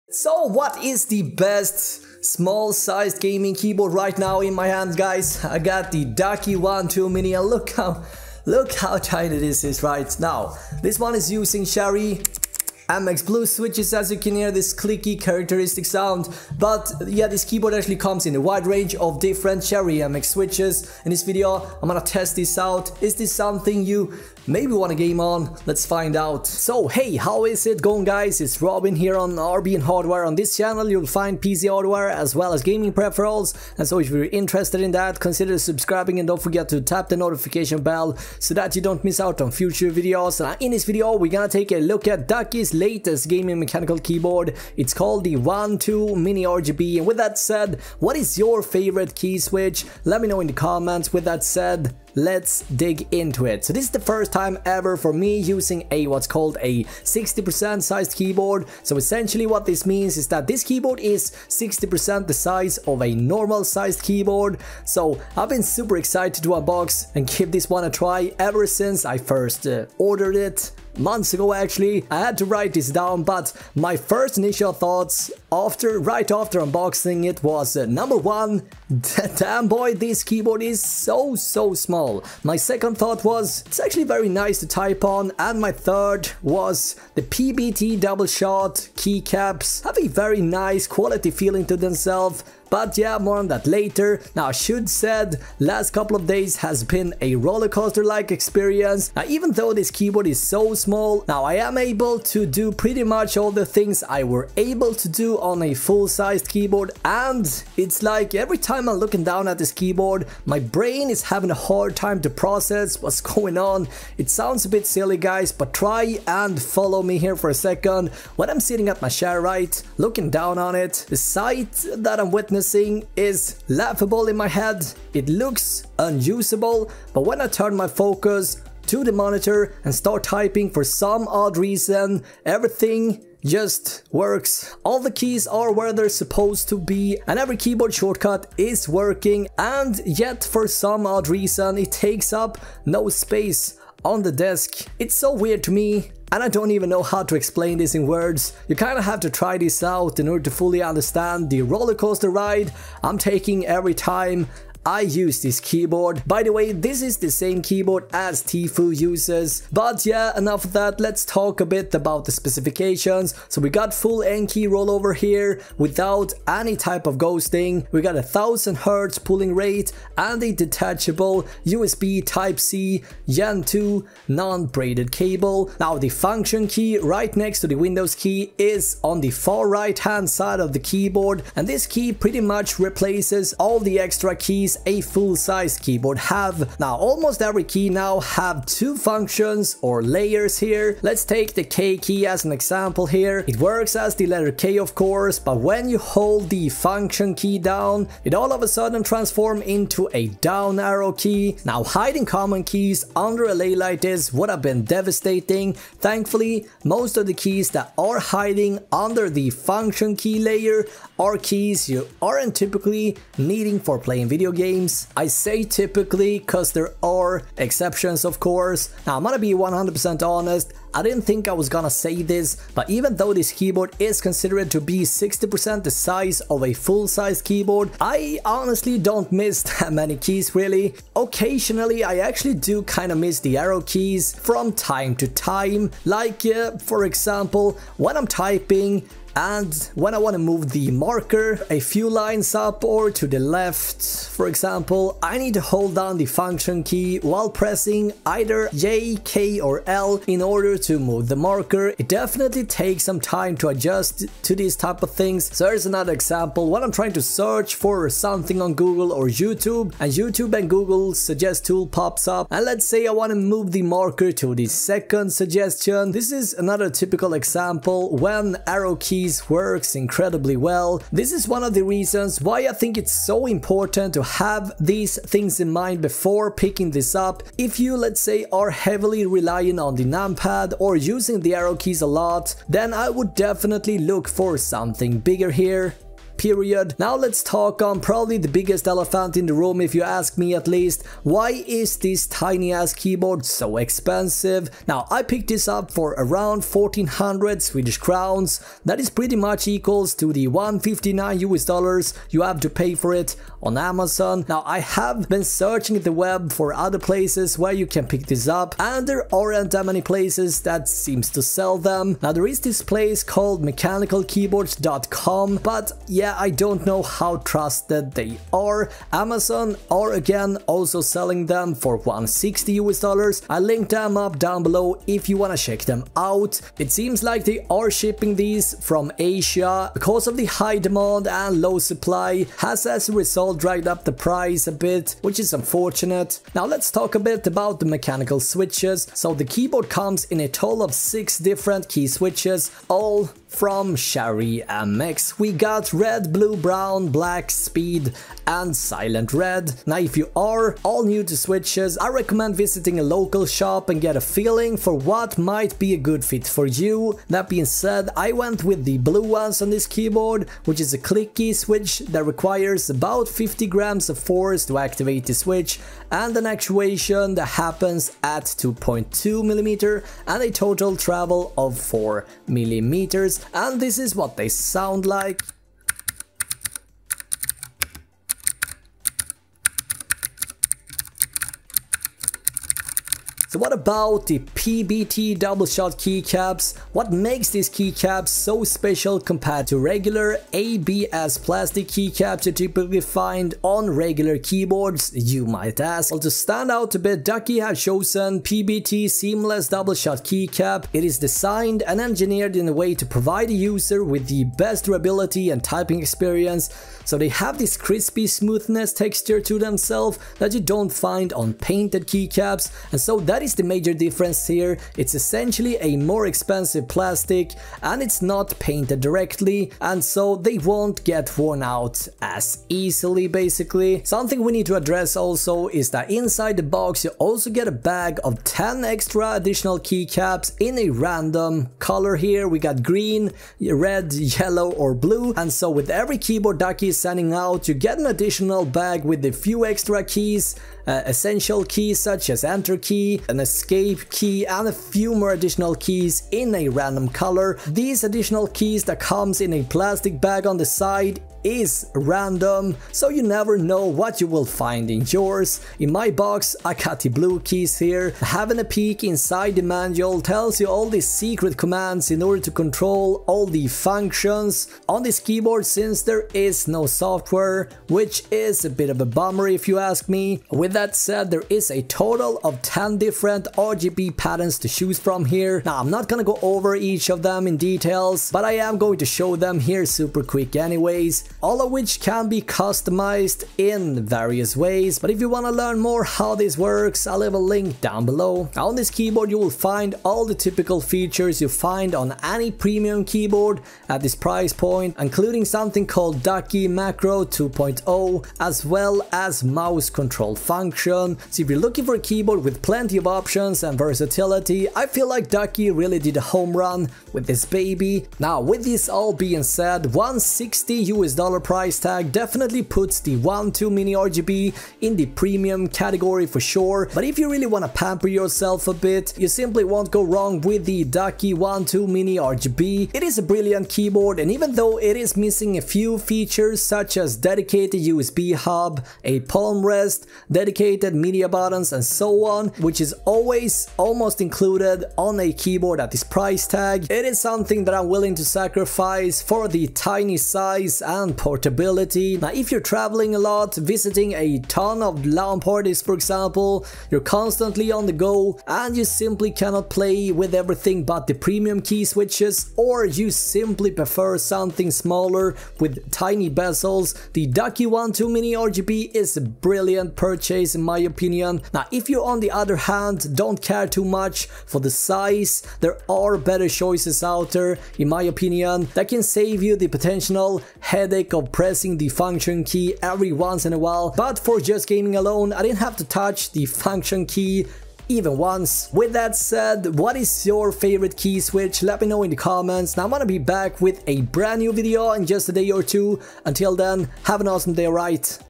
Best 60% Mechanical Keyboard? Ducky One 2 Mini Review (Tfue Keyboard)
is this the best keyboard for fortnite battle royale? This is ducky newest gaming keyboard: one 2 mini RGB, the exact mechanical keyboard gaming pros such as tfue uses. This keyboard is also known as a 60% keyboard, meaning 60 percent the size of a full sized keyboard.

Ducky One 2 Mini RGB

MY 4K SETUP
*CPU* RYZEN 1700
*GPU* DUAL GTX 970
*KEYBOARD* QPAD MK85
*MOUSE* RAZER DEATHADDER
*MOUSEPAD* SteelSeries QcK+

About this video:
In this video, Robin from RBN hardware unboxes and tests and review ducky’s latest and newest gaming mechanical keyboard called one 2 mini rgb and as the name hints, it’s a smaller sized keyboard than what you typically find on the market, only 60% the size of a full sized keyboard.

And although its smaller, no features are sacrificed. Ducky one 2 mini comes in six different key switches: Cherry mx red, blue, brown, silent red, black and speed, all with different characteristics for different playstyles.
One two mini rgb technical details:

Compared to the Fullsize and TKL version,
60% size is more convenient to carry
Function key position rearranged
Smaller size, but no functions sacrificed Supports Ducky Macro V2.0 and Mouse control function

The new bezel design shares a similar sleek frame as it’s predecessor, but the One 2 Mini incorporates dual colors on the bezel to match all varieties of keycap colorways.

$160 – definitely premium, this is also what I payed for it here in Sweden, typically, we here in Sweden always pay about 20-30% more than you ameriacan pay, due to various taxes and such.

To stand out in the crowd we chose to use PBT seamless doubleshot keycaps. It is designed and engineered in a way to provide the user with the best durability and typing experience. Varieties of colorways available.

German Made Key Switches
Cherry MX switches has a lifespan of over 50 million operations. Standard cross-shaped design allows user to replace keycaps with ease.

3 Level Adjustable Feet
The One 2 can be adjusted into three placement angles, optimizing typing comfortability and legend visibility.

Detachable USB Type-C cable provides convenience to users. We use USB HID with the highest frequency of 1000Hz polling rate, meaning the keyboard is sending its input signal(s) to your PC 1000 times per second. This feature minimizes the delay of when the keys are being pressed and signal received by the computer.
USB N-Key Rollover (USB NKRO) and PCB

Under USB, keyboard can handle any number of simultaneous key presses. You can use the keyboard confidently in any situations knowing that there will never be ghosting or key blocking issue. This will give longer life expectancy as well as signal stability of the keyboard.

Specs:
One 2 Mini RGB 60%
-Model Namee: DKON1861ST
-Type: Mechanical Keyboard
-Switch: Cherry MX mechanical switches
-Interface: USB 2.0
-Keycap material: ABS or PBT
-Simultaneous key input: USB N-Key Rollover (NKRO)
-Legend printing: Double-Shot or Laser Engraved
-Dimensions: 302 x 108 x 40 mm
-Weight: 590g